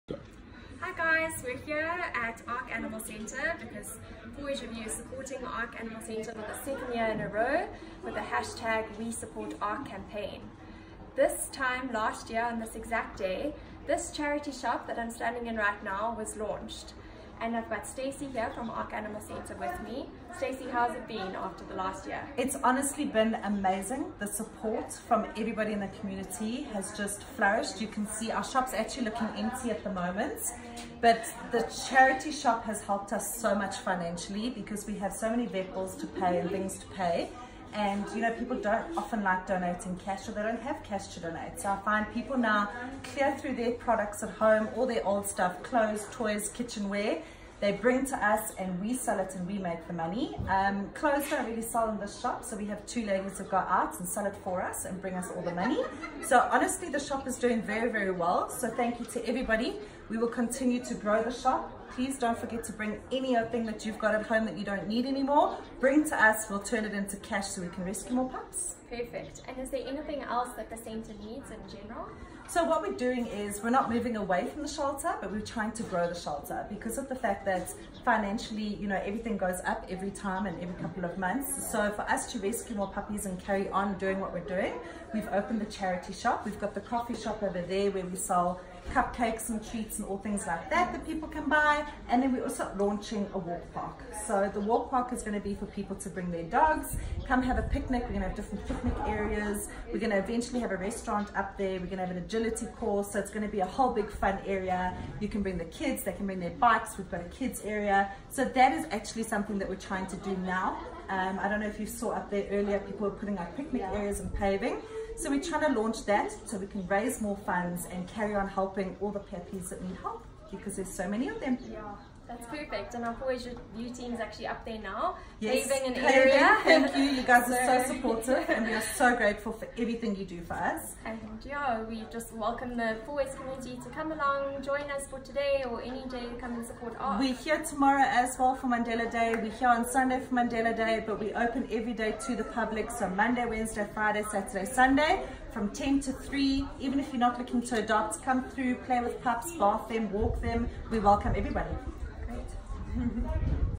Ark Animal Centre's charity shop celebrates first anniversary
Caxton Local Media's Joburg North branch group editor, Daniella Potter chats to Stacey from Ark Animal Centre about the success of the centre's charity shop, Ark's Attic.